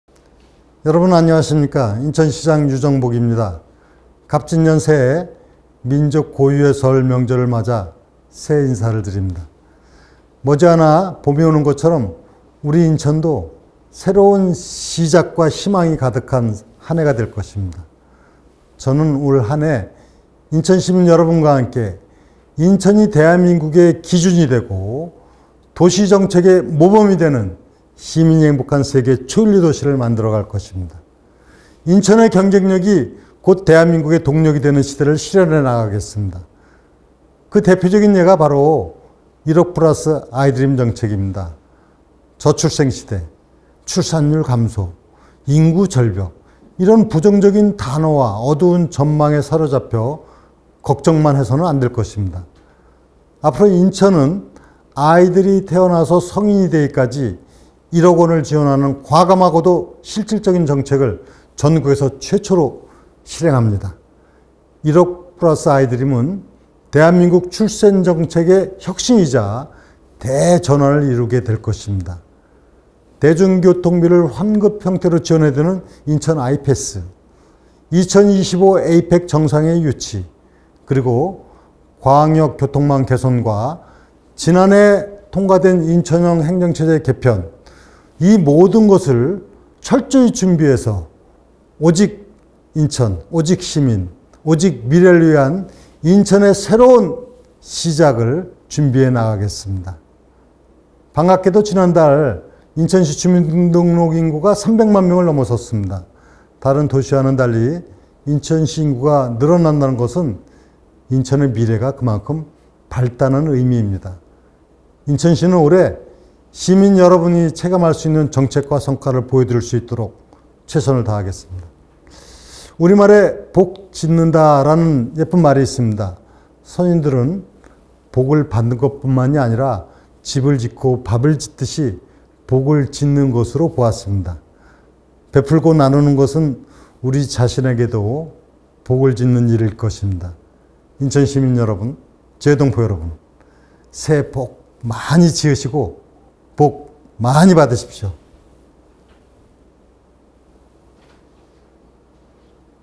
여러분 안녕하십니까? 인천시장 유정복입니다!
갑진년 새해, 민족 고유의 설 명절을 맞아
새해 인사를 드립니다.

머지않아 봄이 오는 것처럼
우리 인천도 새로운 시작과 희망이 가득한
한 해가 될 것입니다.

저는 올 한해 인천시민 여러분과 함께
인천이 대한민국의 기준이 되고 도시 정책의 모범이 되는
시민이 행복한 세계초일류도시를 만들어 갈 것입니다.
인천의 경쟁력이 곧 대한민국 동력이 되는
시대를 실현해 나가겠습니다.

그 대표적인 예가 바로 1억 플러스 아이드림 정책입니다.
저출생시대, 출산율 감소, 인구 절벽
이런 부정적인 단어와 어두운 전망에 사로잡혀
걱정만 해서는 안 될 것입니다.

앞으로 인천은 아이들이 태어나서 성인이 되기 전까지
1억원을 지원하는 과감하고도 실질적인 정책을
전국에서 최초로 실행하고자 합니다.
1억 플러스 아이드림은
대한민국 출생정책의 혁신이자, 대전환을 이룰 것입니다.

대중교통비를 환급형태로 지원해드리는 인천 I-패스,
2025 APEC정상회의 유치, 광역교통망 개선,
그리고 지난해 통과된 인천형 행정체제 개편과
그에 따른 준비 과정 모두
오직 시민, 오직 인천, 오직 미래를 위한
인천의 새로운 시작이 될 것입니다.

반갑게도 지난달 인천시 주민등록 인구가 300만을 넘어섰습니다.
다른 도시와는 달리 인천시 인구가 늘어난다는 것은 뜻깊은 일입니다.

여러분, 인천시는 올해 시민 여러분이 체감하실 수 있는
정책과 성과를 보여드릴 수 있도록 최선을 다하겠습니다.
우리 말에 ‘복 짓다’라는 예쁜 말이 있습니다.
선인들은 복을 받는 것뿐만 아니라
집을 짓고 밥을 짓듯이 복을 짓는 것으로 보았습니다.
베풀고 나누는 것은 우리 자신에게도 복을 짓는 일일 것입니다.

인천시민 여러분, 재외동포 여러분
새해 복 많이 지으시고, 복 많이 받으십시오.

- 인천광역시장 유정복 드림 -